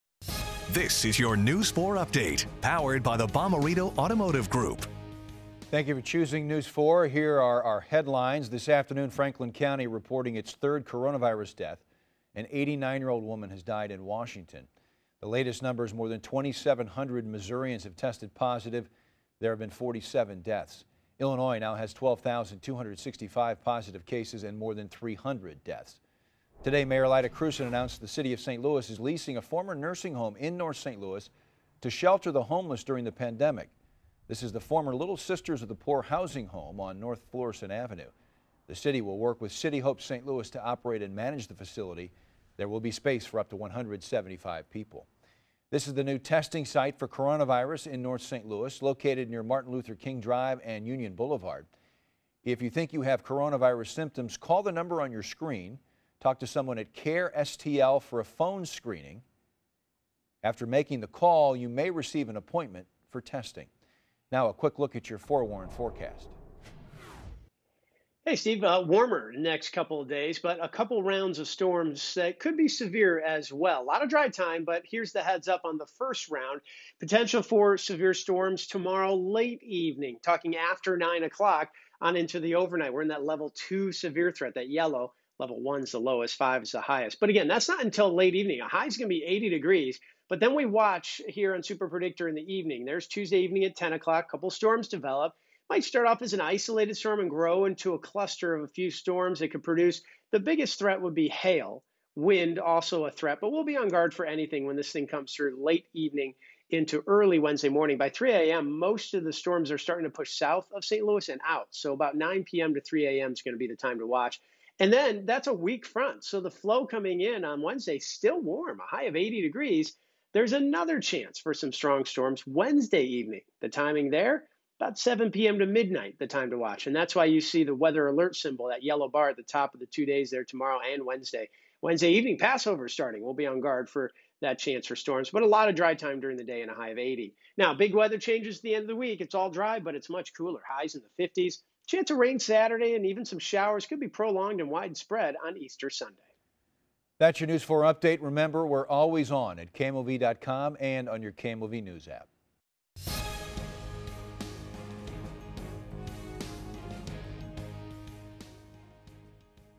News 4 Update: April 7,2020
One month since the first case reported in the state, the latest statewide COVID case numbers and much more. Stay updated with the newest flash briefing